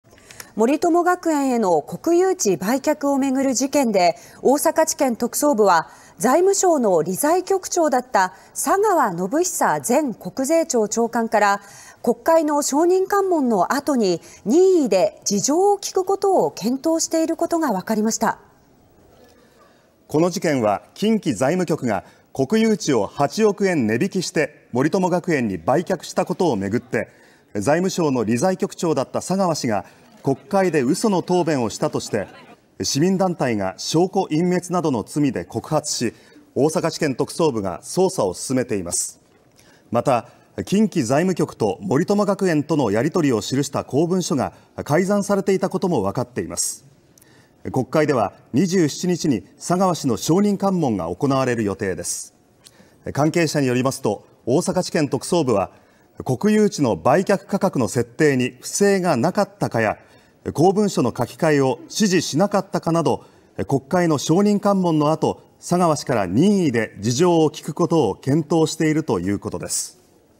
証人喚問後に佐川氏の聴取を検討 大阪地検特捜部(18/03/21)
森友学園への国有地売却を巡る事件で、大阪地検特捜部は財務省の理財局長だった佐川宣寿前国税庁長官から国会の証人喚問の後に任意で事情を聴くことを検討していることが分かりました。